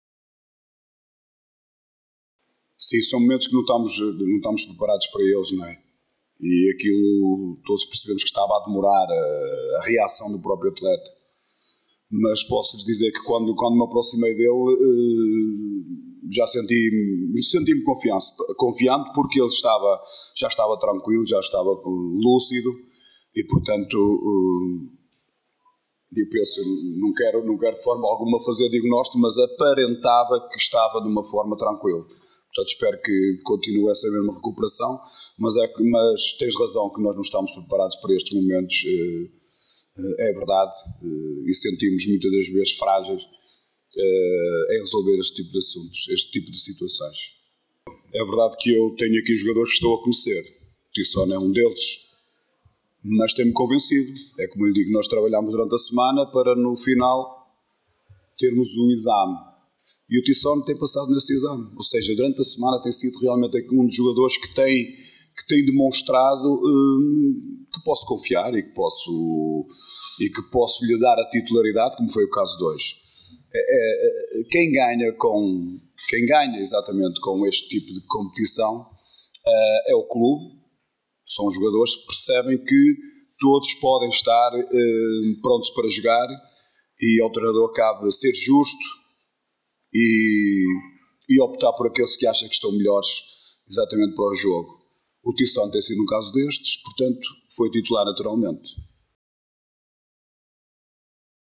Tissone: a apreensão e o elogio do treinador
A propósito dos momentos de tensão que se viveram por causa do estado de Tissone, José Mota, treinador do Clube Desportivo das Aves​, reconheceu não ser fácil lidar com a situação, mas disse estar confiante na recuperação do jogador. Depois, elogiou a sua prestação, no jogo e na semana que o antecedeu